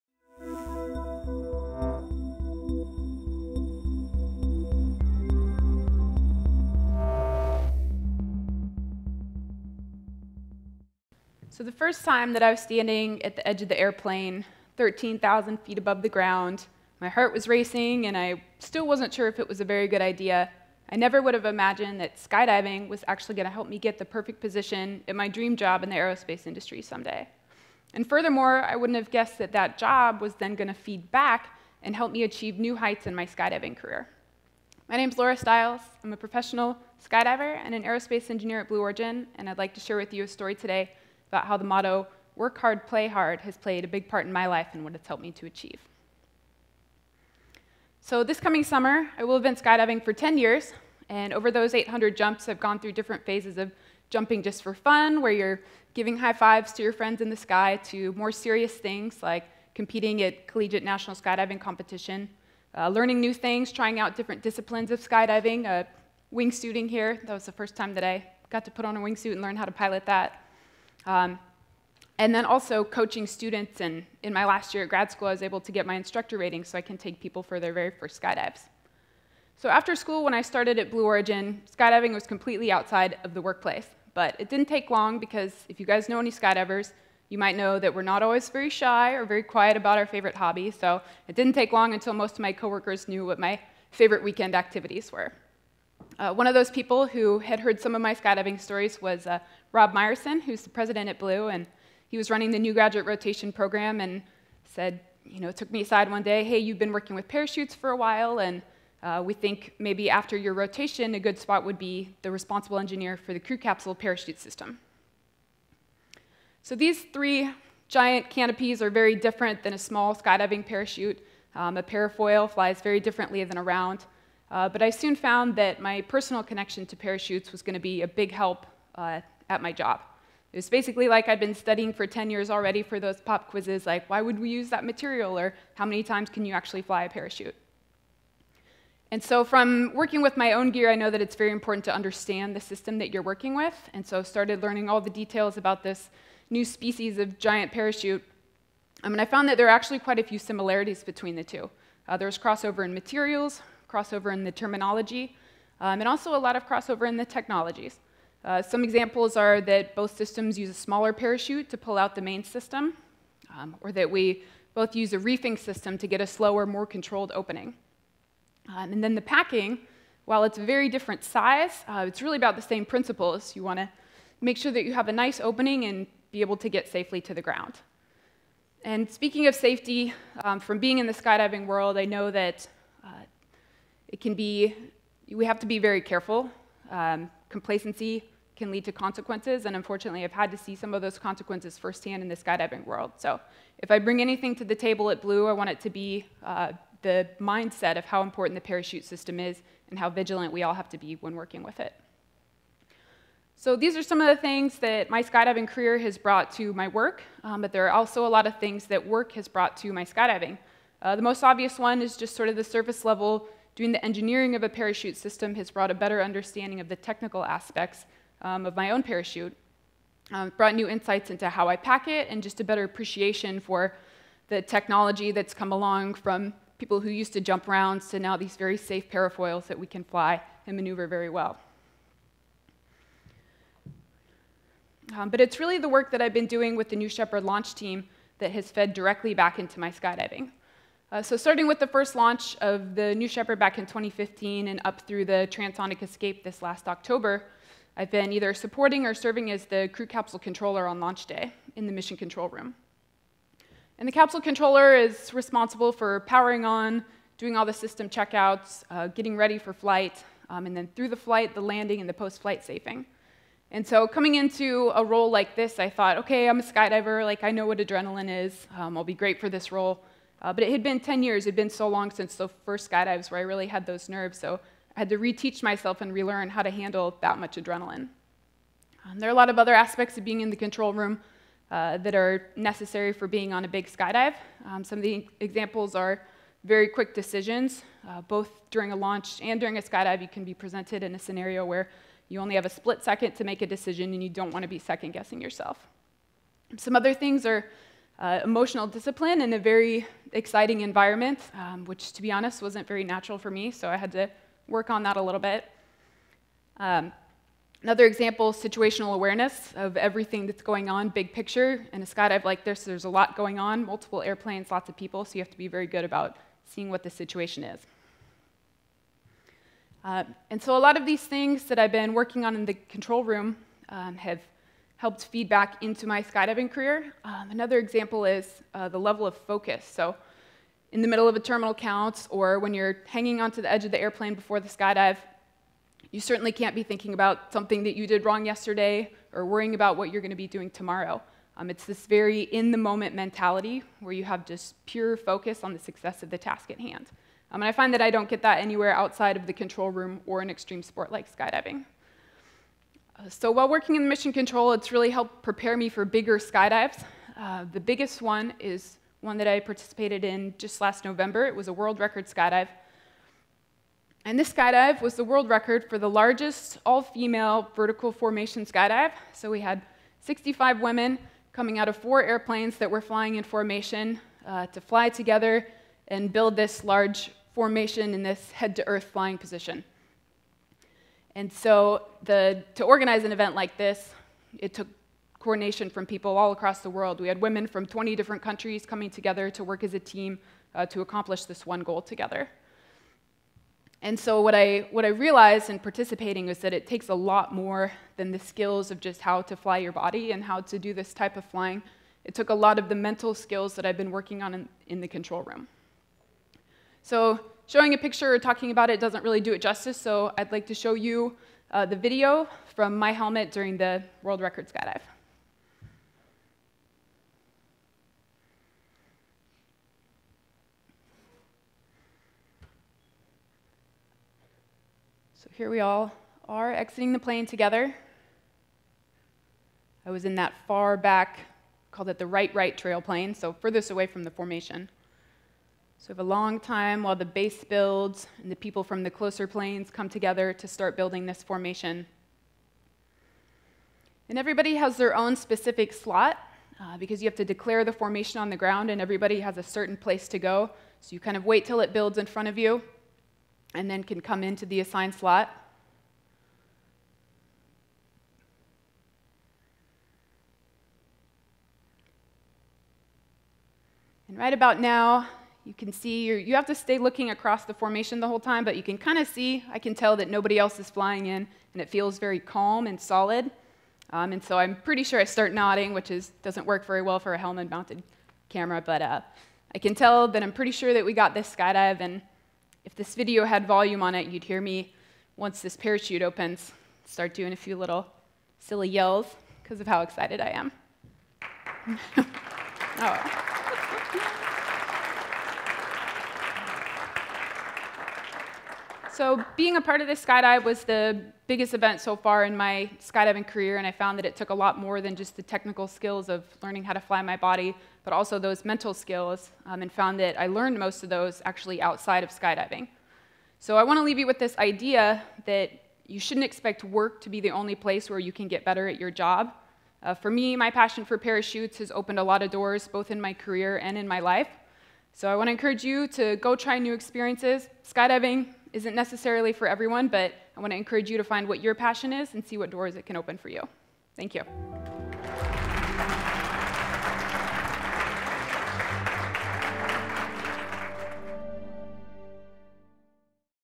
SGx2017 - Laura Stiles - Work Hard, Play Hard: my Passion for Parachutes
SGx brought together young professionals, experts in and government leaders from the space industry to discuss pressing issues and innovative ideas through fast-paced lightning talks. No ties. No panels. No business pitches.